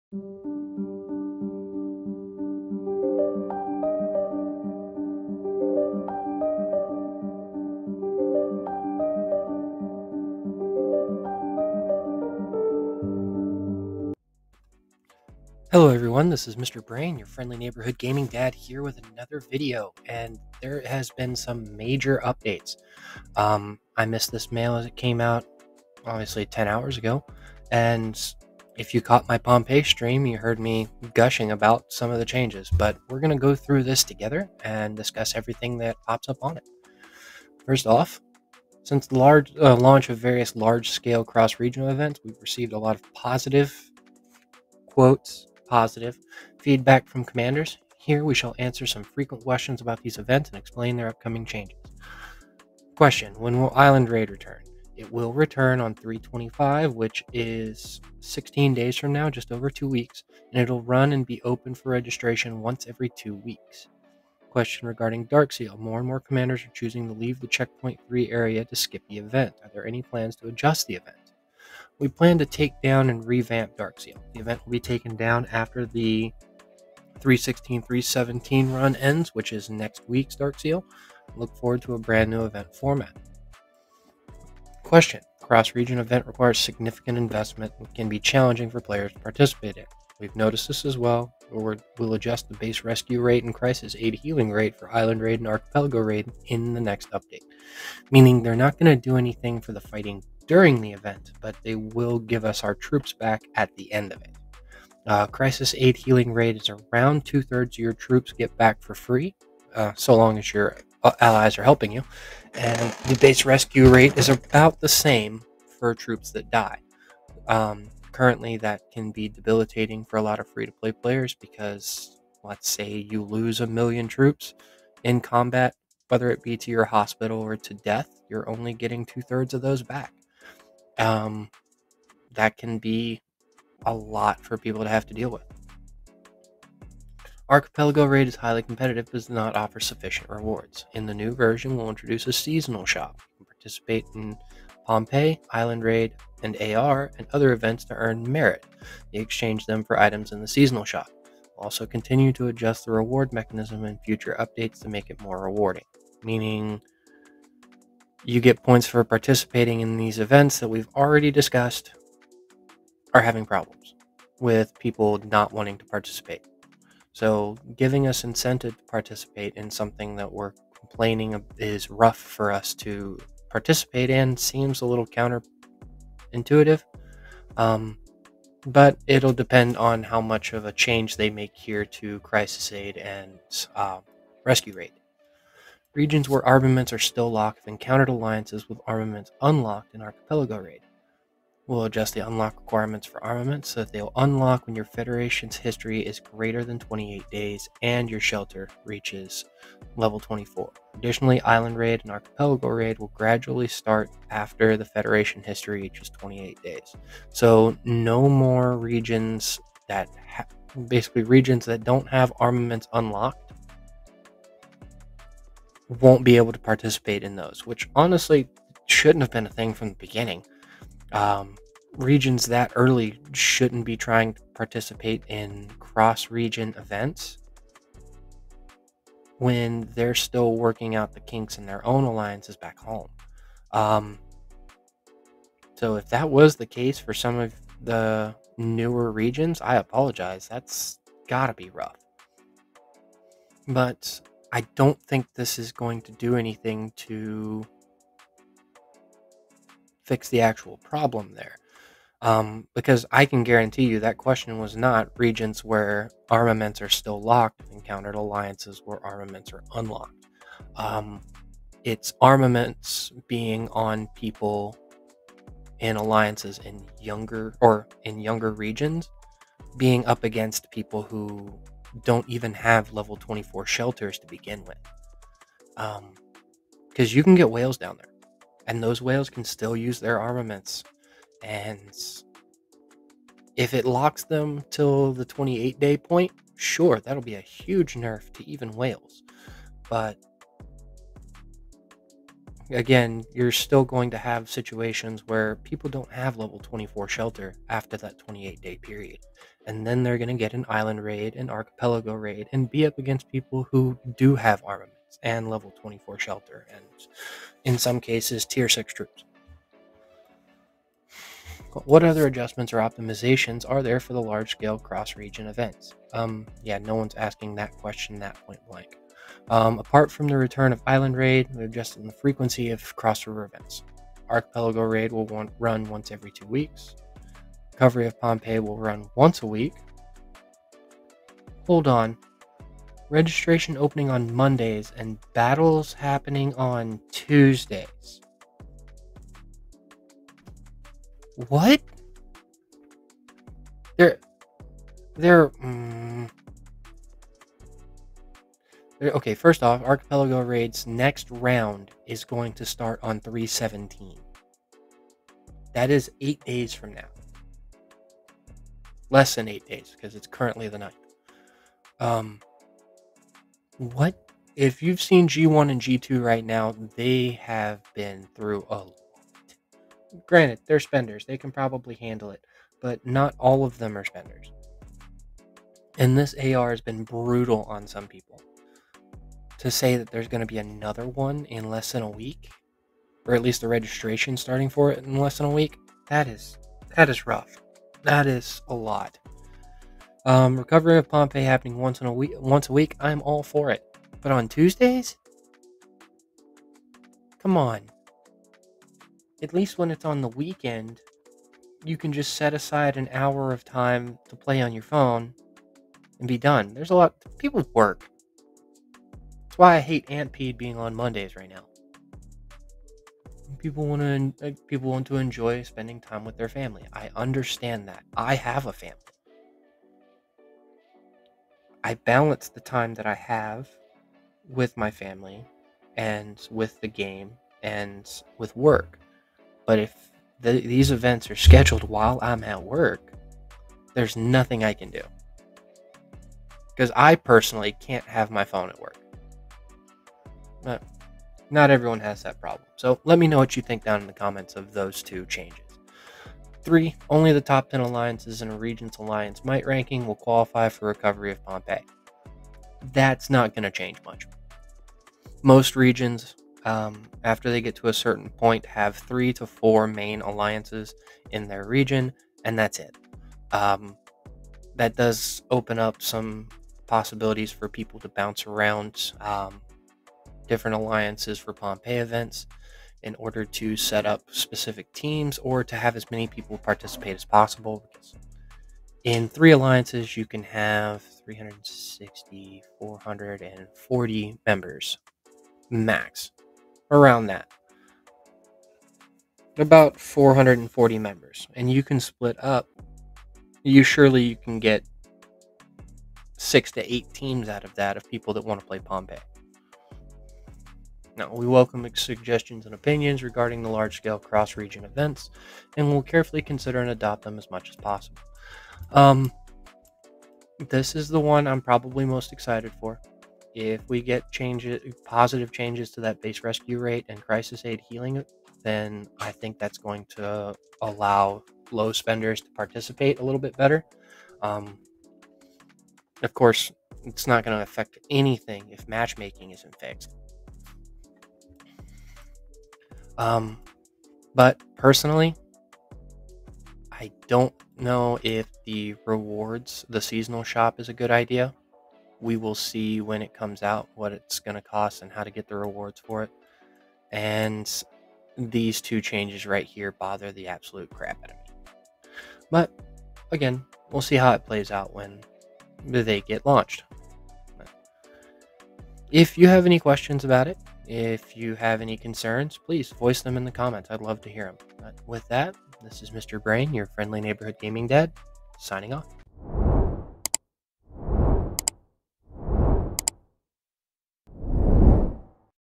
Big Updates 03/09/2024
alot of cross region event changed. let me know what you think about them in the comments.